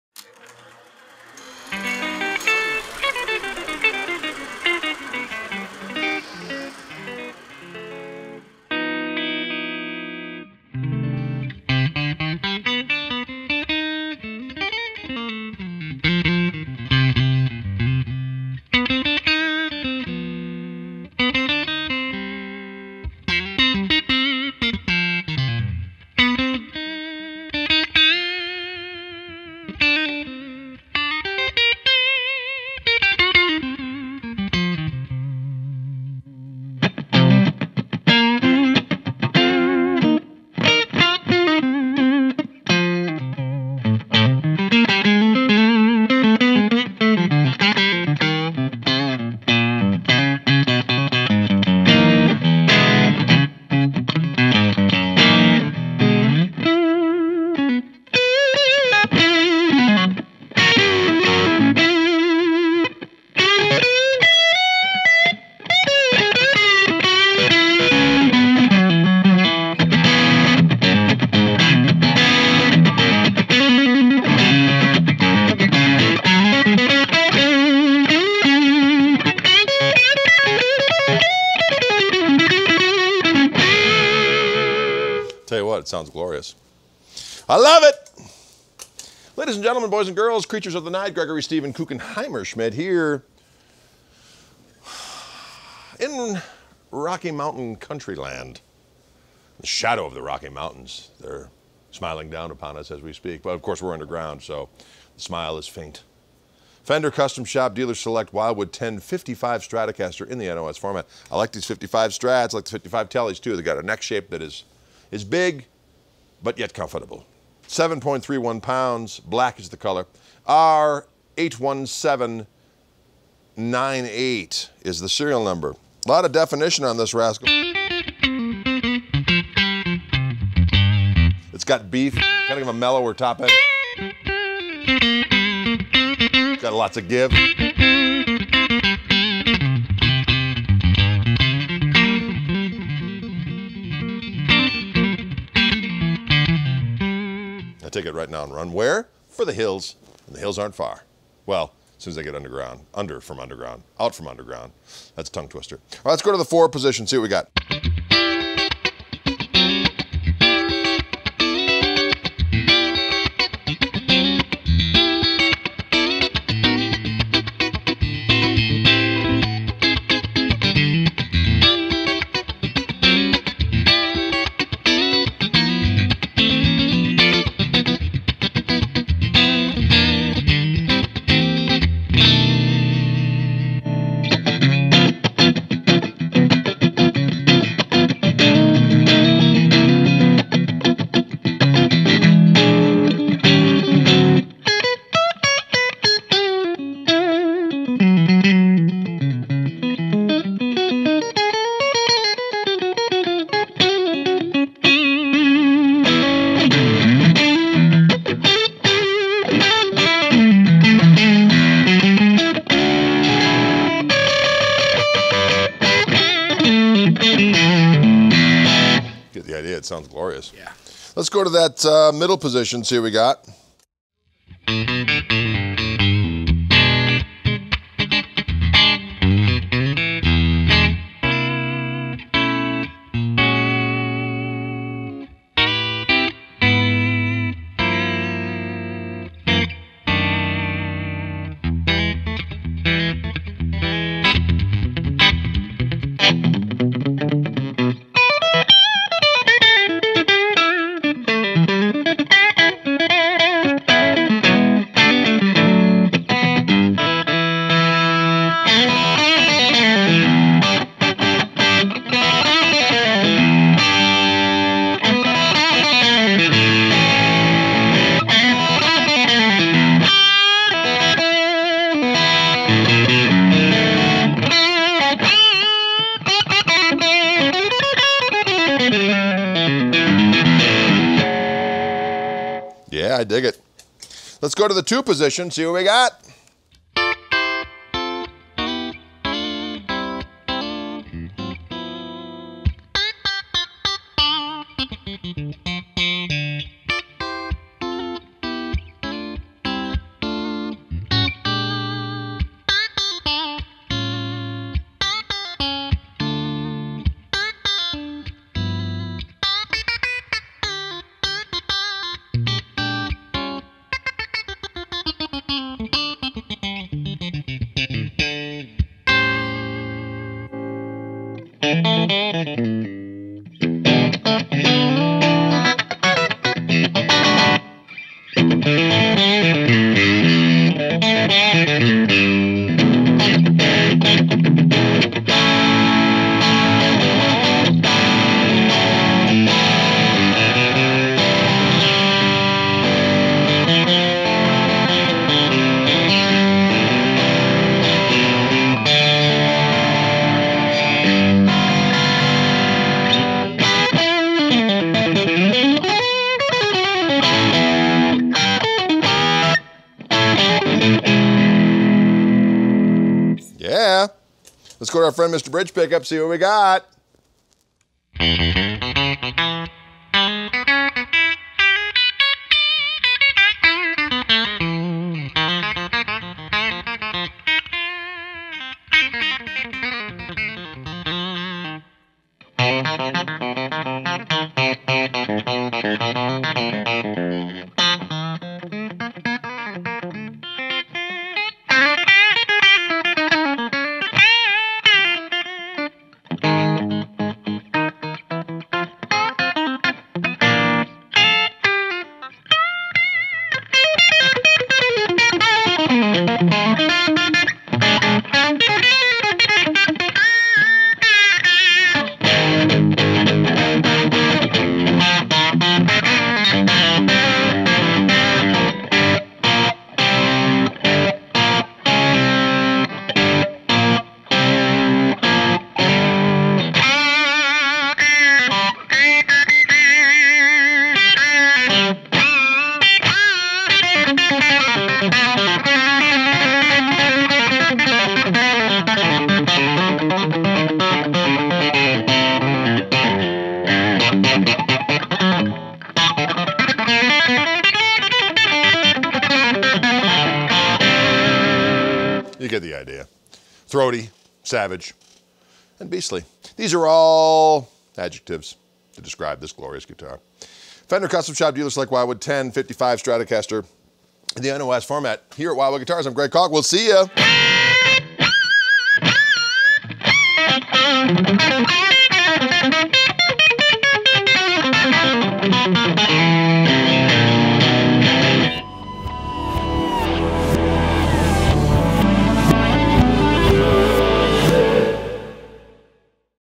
Fender Custom Shop Dealer Select Wildwood "10" 1955 Stratocaster NOS • SN: R81798
The latest in our long, illustrious lineage of killer Wildwood 10's have arrived. These sonic sledgehammers have a monster tone, with a killer vibe. If you've dreamed of a guitar that has the soul of a vintage guitar and the ergonomics of a modern guitar, your dream has come true! They are only available at Wildwood Guitars, and we are mighty proud of these creations. ""Come and feast!"", Greg Koch often says when demonstrating these delectable morsels of tonal savagery.

Serial Number: R81798
Brand: Fender Custom Shop
Model: Dealer Select Wildwood "10" '55 Stratocaster
Type: Stratocaster
Year: 1955
Finish Color: Black-Tone Sunburst
Aging: NOS
Finish Type: Nitrocellulose Lacquer
Weight: 7.31 lbs.
Body Wood: Ash
Neck Wood: Quarter Sawn Maple
Neck Shape: '54 Soft "V"
Neck Dimensions: .880 1st - .980 12th
Fingerboard: Maple
Fingerboard Radius: Exclusive Wildwood Custom "10" Radius
Inlays: Dot
Scale Length: 25.5"
Width at Nut: 1.650"
Nut Material: Bone
Frets: Dunlop 6105
Pickups: 3 John Cruz Limited Single-Coils
Controls: 1 Volume, 2 Tone, 5-Way Switch
Hardware : Nickel
Bridge: Vintage Tremolo
Tuners: Vintage Style with Nickel Buttons
Pickguard: Single-Ply Parchment
COA: Yes